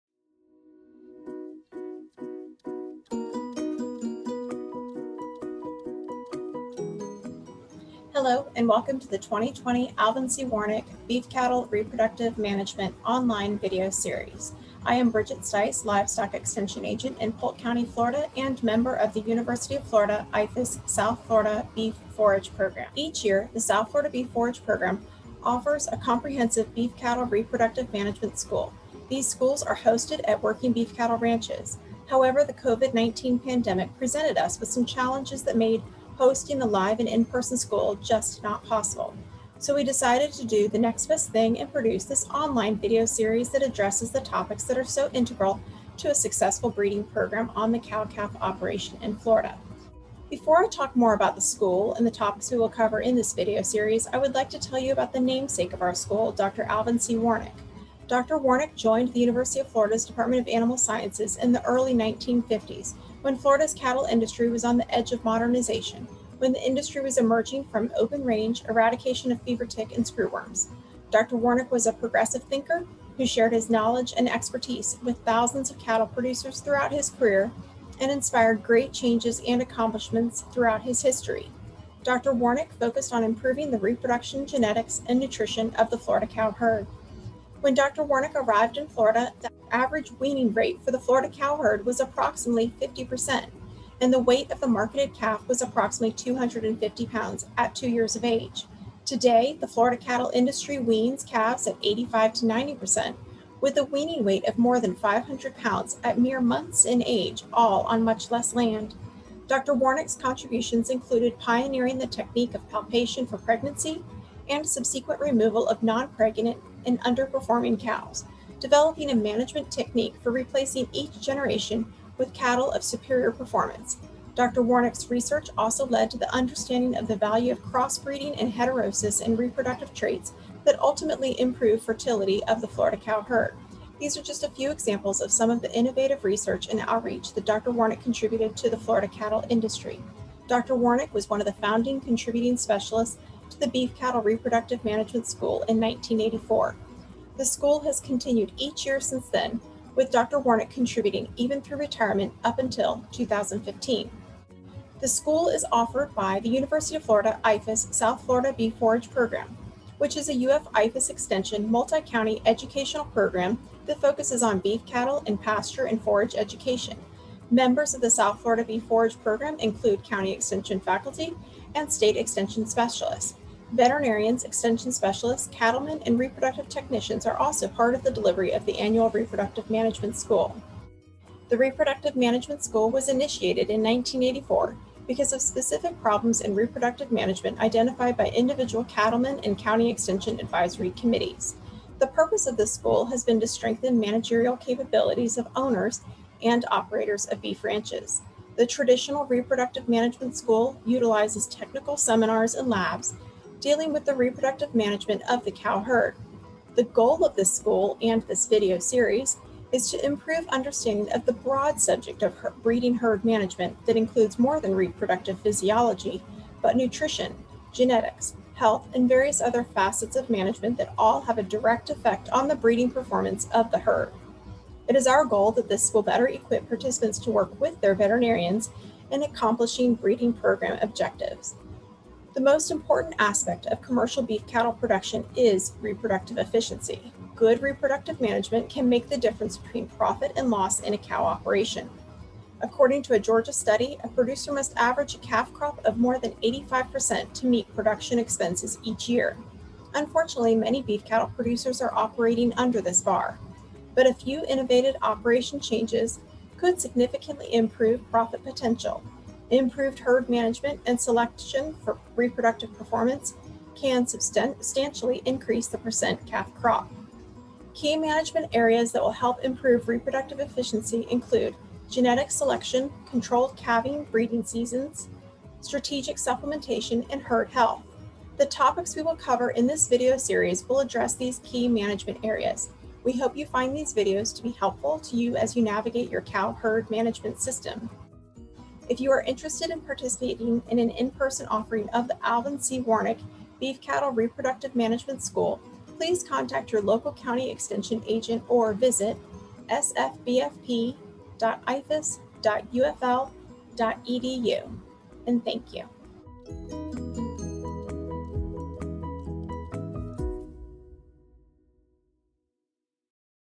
An Introduction to Beef Cattle Reproductive Management and the ACW Reproductive Management School
Video 1 of 14 of the Alvin C. Warnick Beef Cattle Reproductive Management Video Series, presented by Bridget Stice.